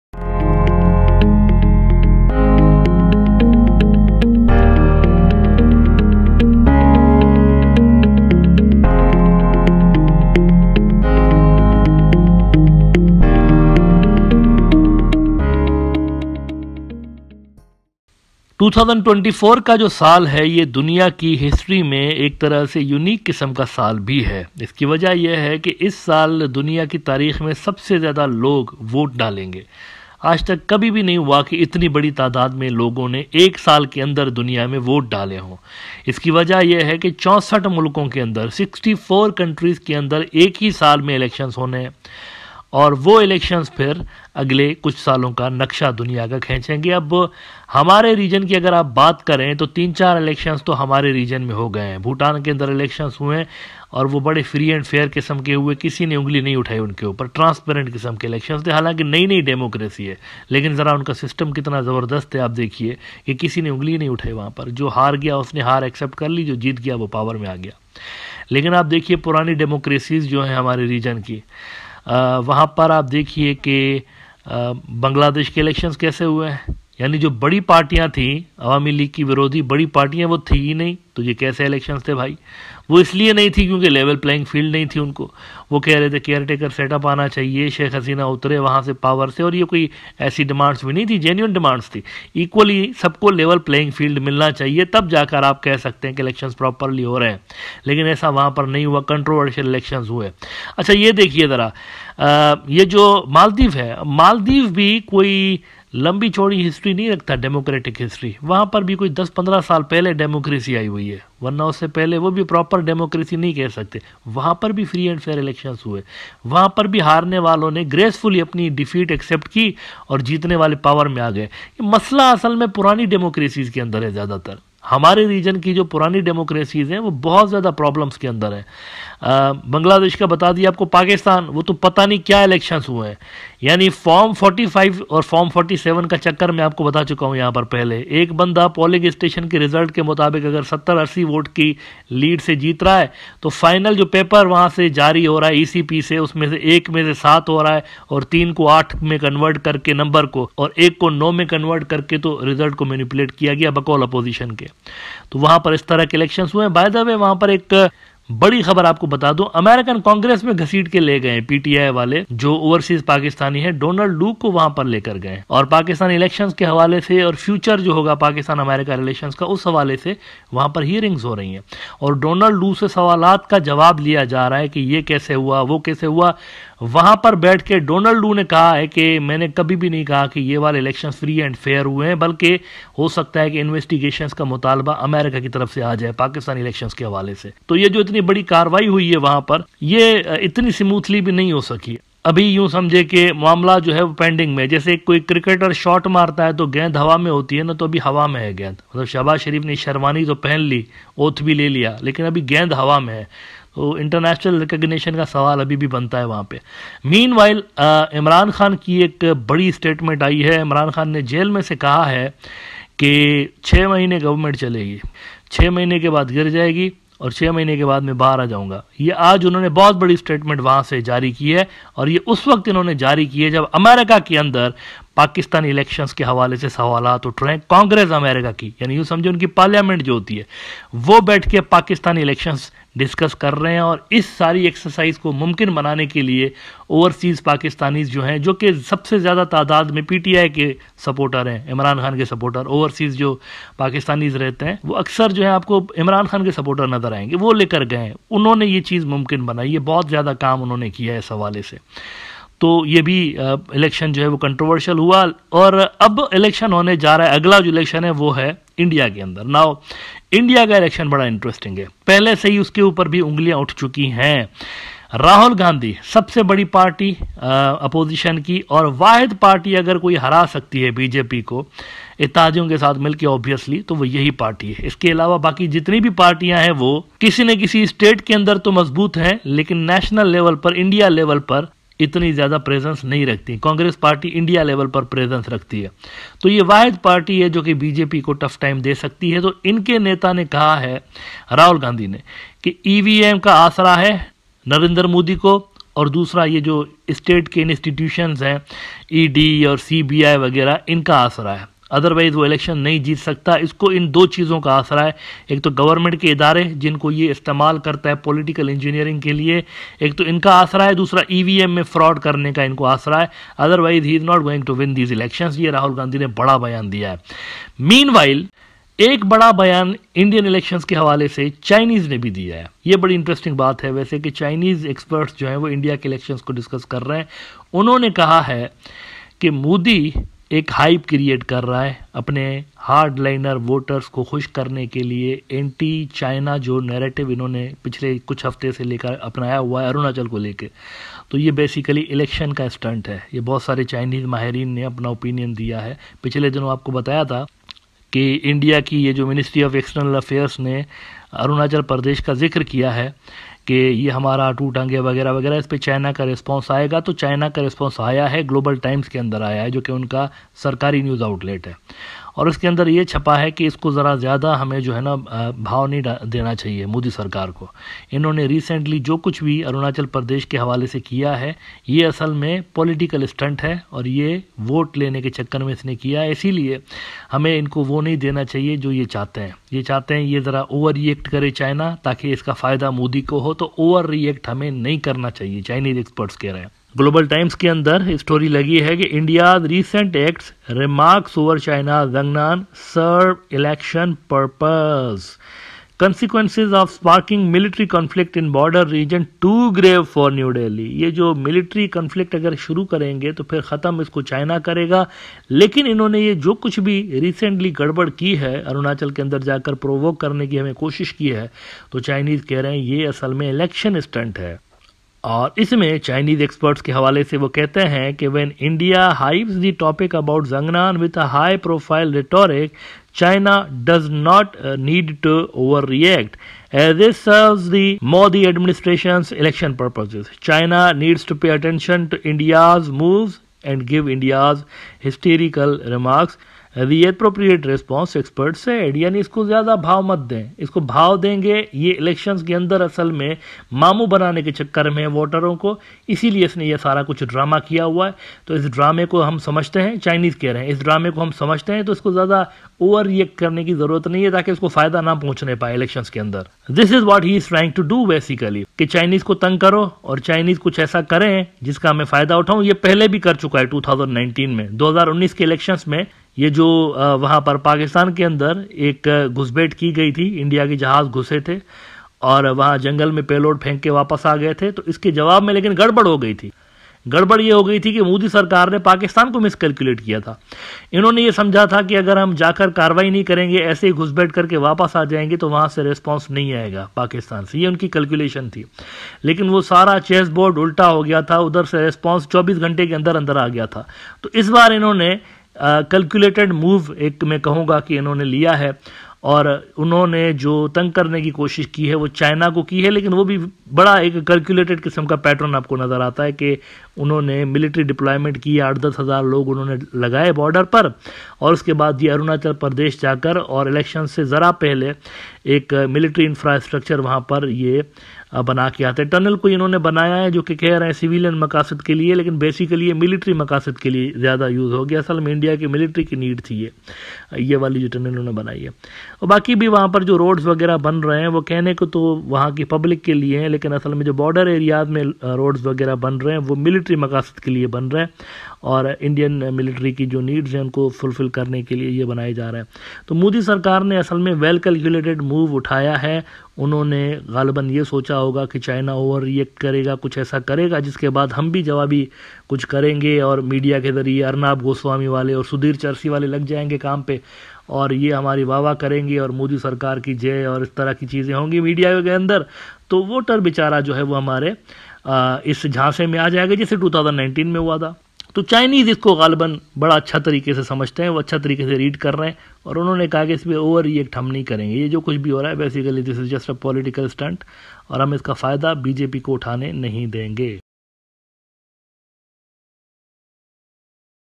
"We know what Modi is trying to do" : Chinese experts | South Asian Elections
Chinese experts believe recent visit of modi in arunachalpradesh was actually a political stunt and Modi wants to provoke China to get political benefits during upcoming elections. meanwhile Imran Khan has sent important message from Jail.